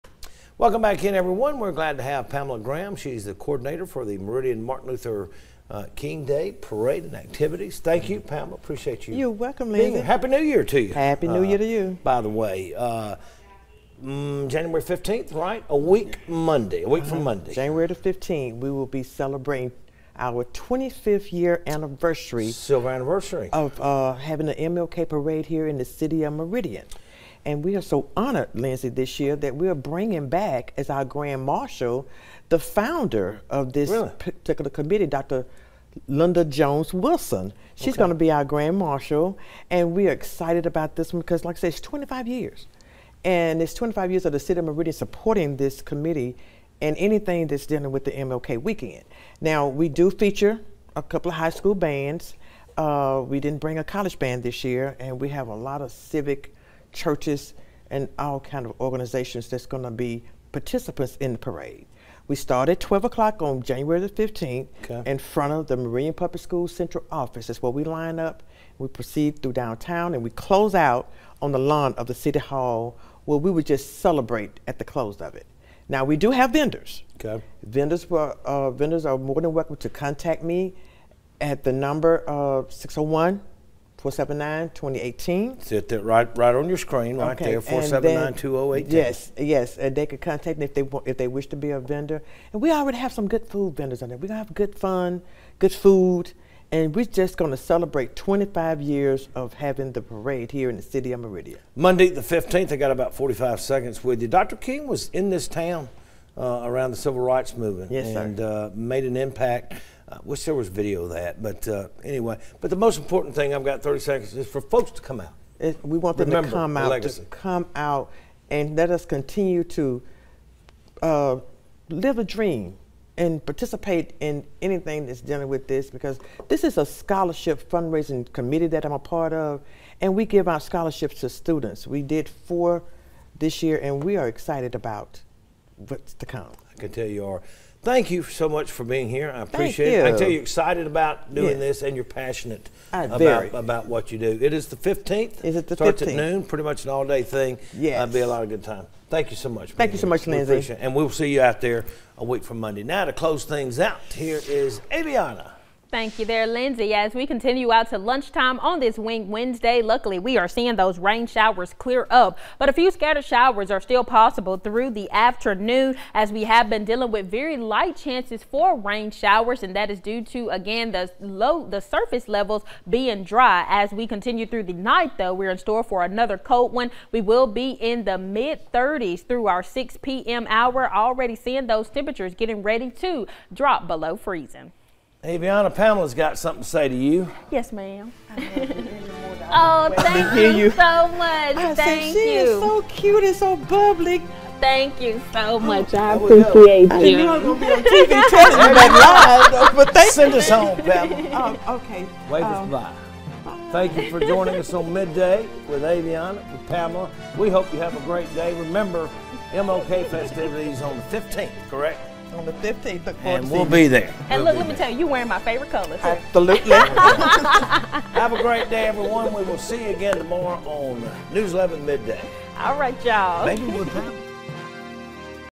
Plans being made for 25th Annual MLK Parade & Celebration in Meridian Jan. 15
The parade starts at 12 noon Monday, Jan. 15.